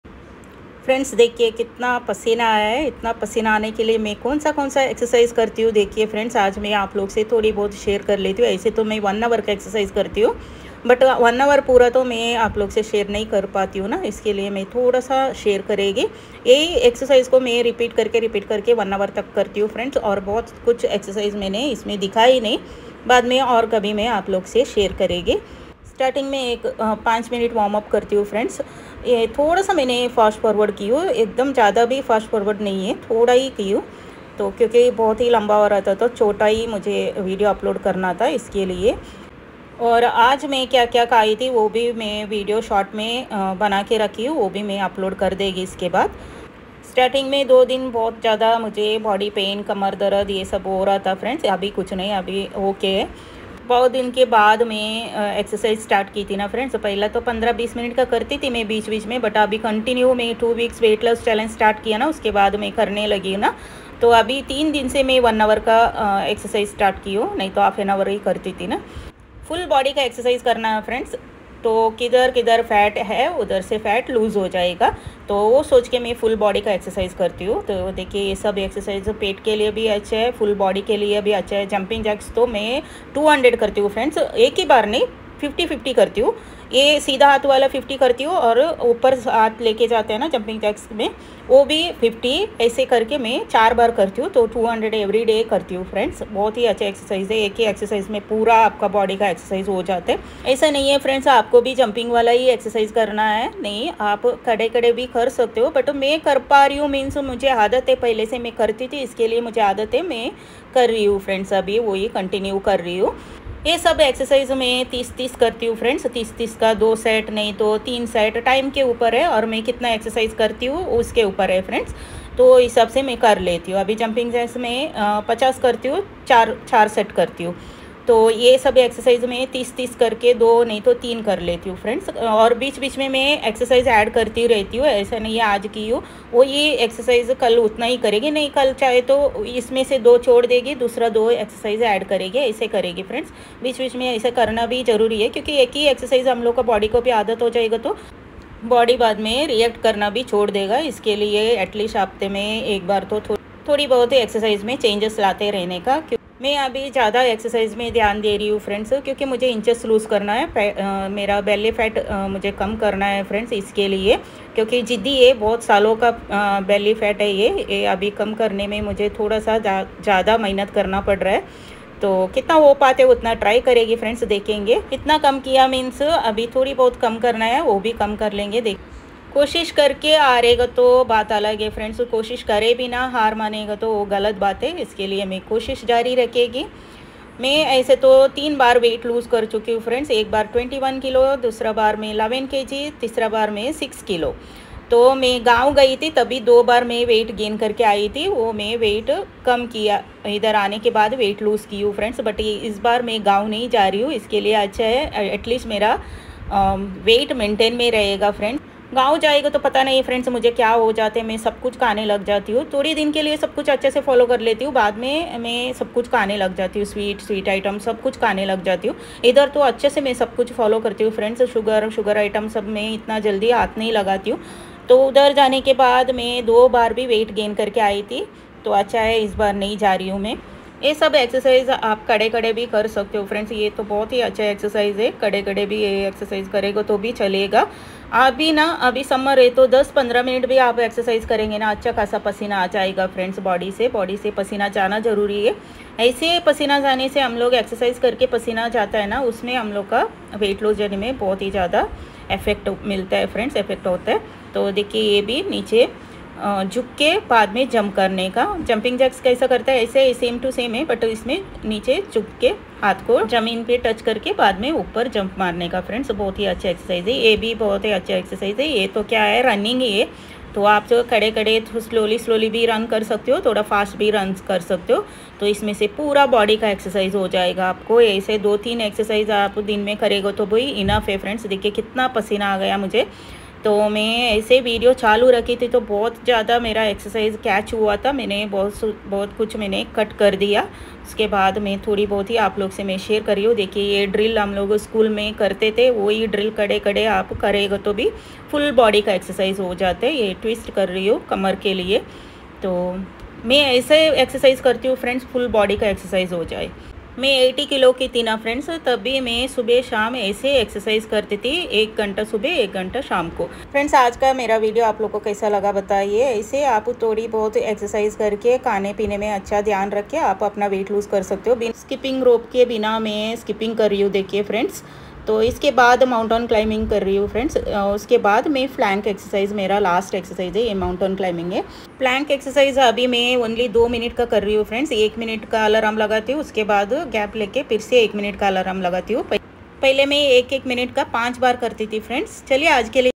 Housewife’s 1 hour full body exercise routine …aab hoga belly fat loss weightlossjourney ￼
Housewife’s 1hour full body jumping exercise routine..Aab hoga belly fat loss